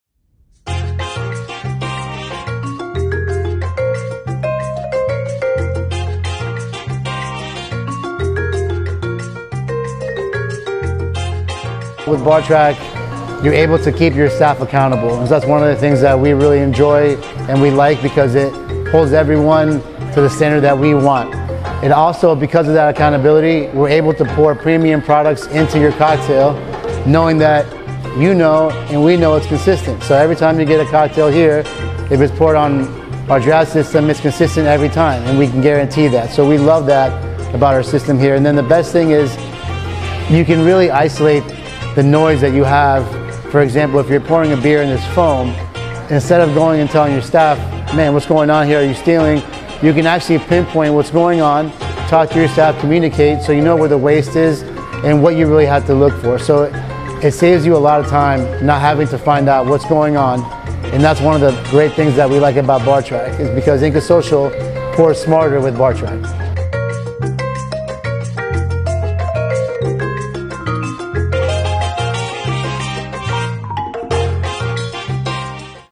Inca Social Pours Smarter™ with BarTrack
“With BarTrack, you’re able to keep your staff accountable so that we all maintain our high standards for quality. This allows all our beverages on draft to remain consistent and deliver that excellent guest experience for every pour. I would absolutely recommend BarTrack.” 💡
- Fito Garcia, Co-Founder and Owner, Inca Social